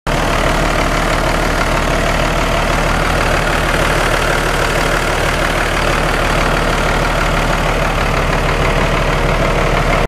2008 PETERBILT 386 For Sale
2008 PETERBILT 386. 15 L CUMMINS ISX WITH 485 HP, JAKE, SLIDING 5TH, 306868 MILES, EATON FULLER 13 SPD TRANS, ALUMINUM WHEELS ALL AROUND WITH 80% RUBBER. SHIPPING AVAILABLE.